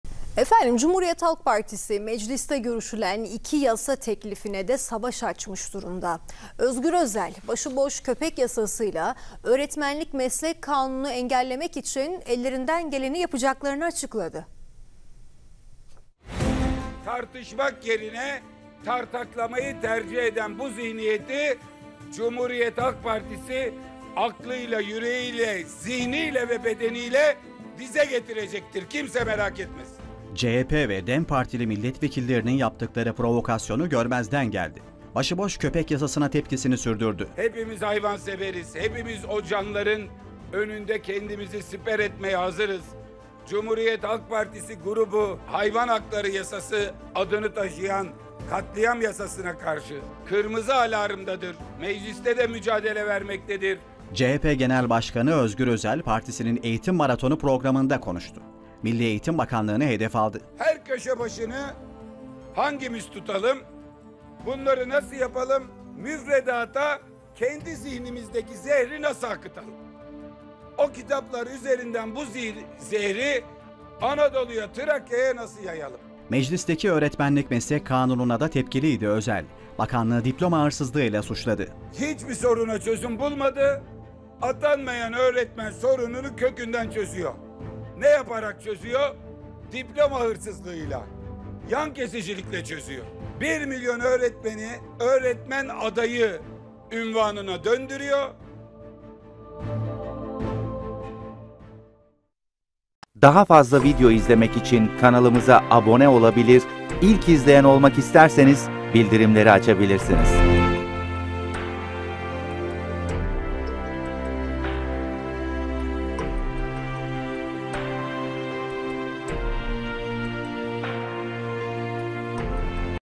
CHP Başıboş Köpek Çözümünde Tarafını Belli Etti!
CHP Genel Başkanı Özgür Özel, başıboş köpek sorununun çözümünde tarafını belli ederek kendimizi siper ederiz çıkışında bulundu.

Türksat 4A
Frekans : 12.103
Sembol: 8333
Polarizasyon: Yatay ( Horz )
Modulasyon: 8SPK